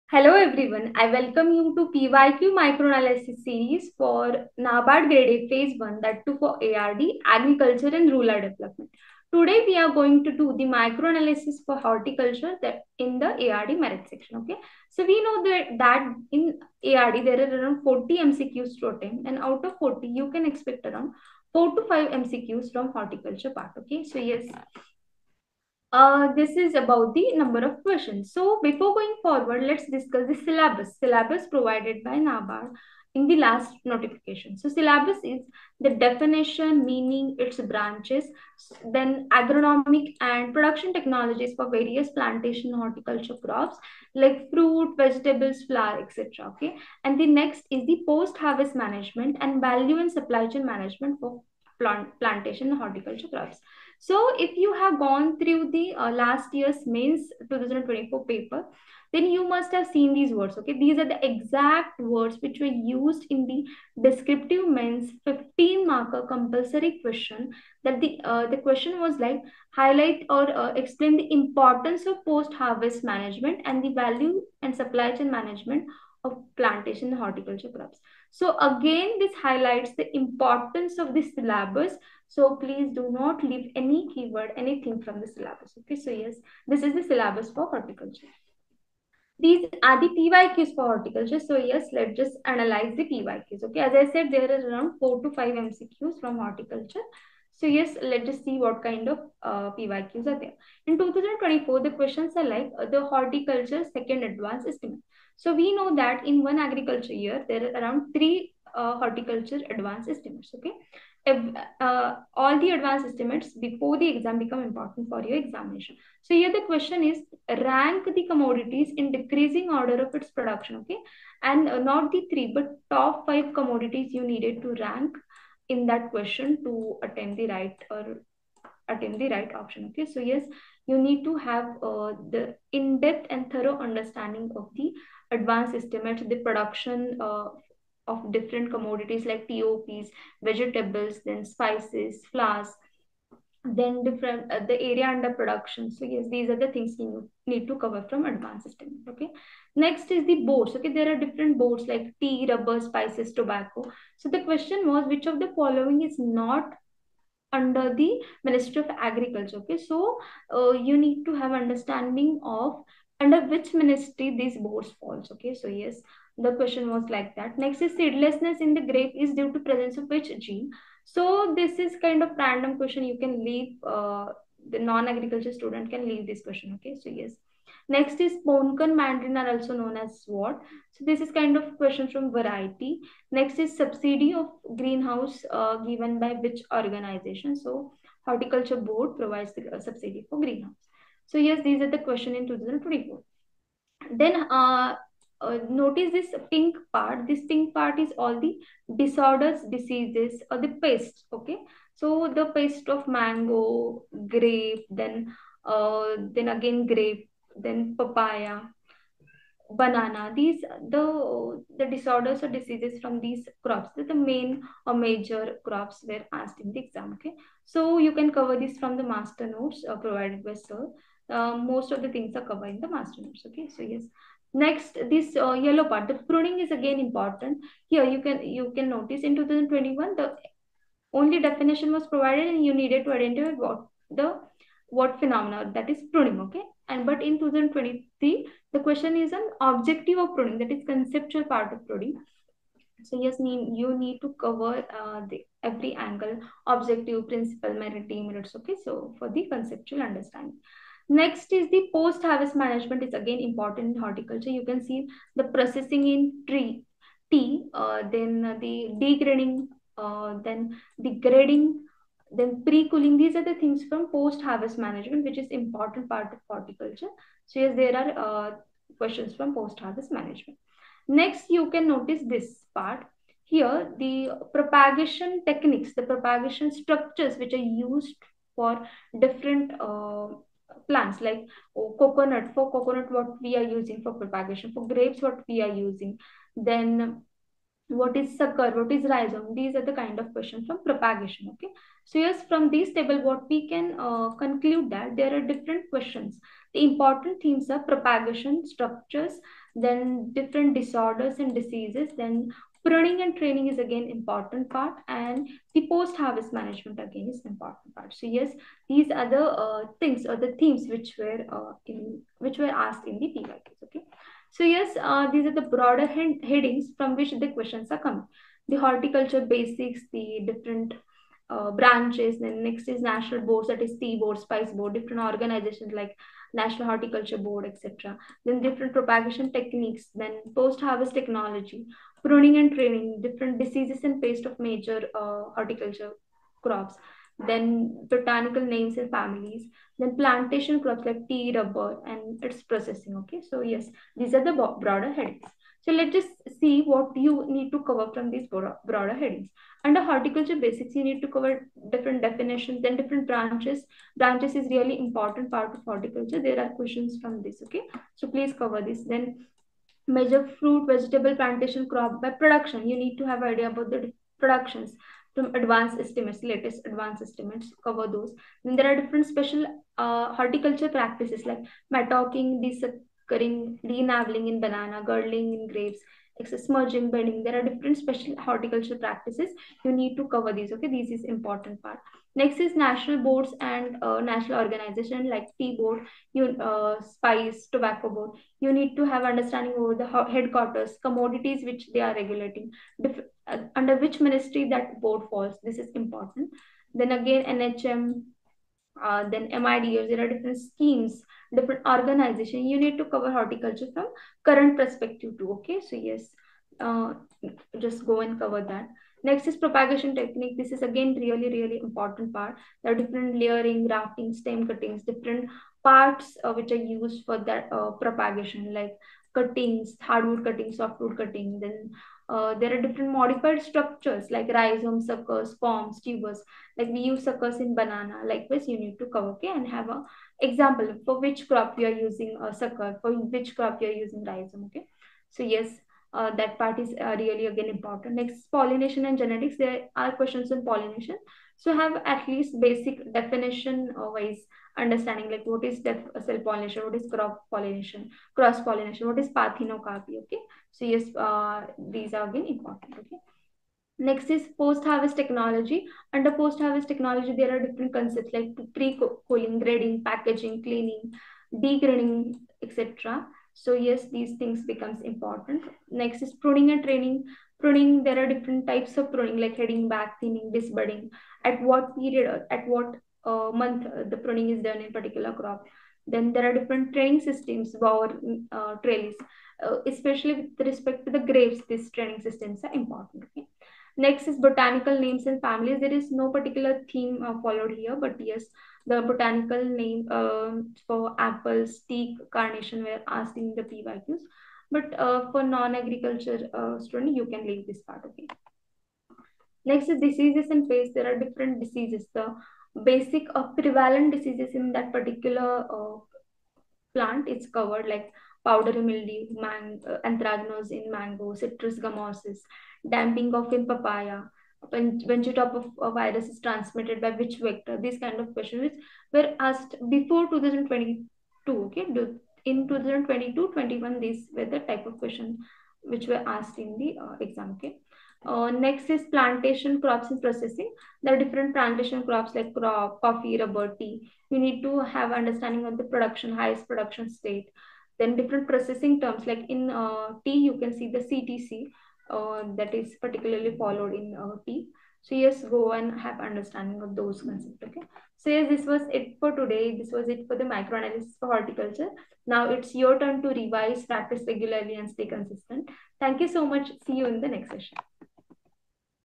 NABARD Grade A PYQ Analysis | Horticulture | Analysis 2021-2024
🎯 NABARD Grade A PYQ Analysis | Horticulture (2021–2024) | Clarity4Sure Mentorship

In this detailed session, we analyze Horticulture-based PYQs (Previous Year Questions) from NABARD Grade A 2021 – 2024 to help you decode the trend, weightage, and recurring concepts for the NABARD Grade A 2025 Exam.
Understand what types of questions are repeatedly asked, which sub-topics hold the maximum marks, and how to prepare smartly in the final 45-day phase!

✅ Topic-wise analysis of PYQs (2021–24) — Fruits, Vegetables, Plantation, and Floriculture

✅ High-yield horticulture concepts for NABARD 2025

✅ Key schemes, missions & crop-specific focus areas

✅ Strategy for integrated ARD + ESI coverage

✅ How to align PYQs with the latest syllabus trends

💡 Useful Links

Learn the pattern & depth of questions NABARD asks in Horticulture.

Identify static + applied portions from PYQs.

Strengthen conceptual clarity for descriptive & objective sections.

Perfect resource for aspirants under Mission NABARD 2025 by Clarity4Sure.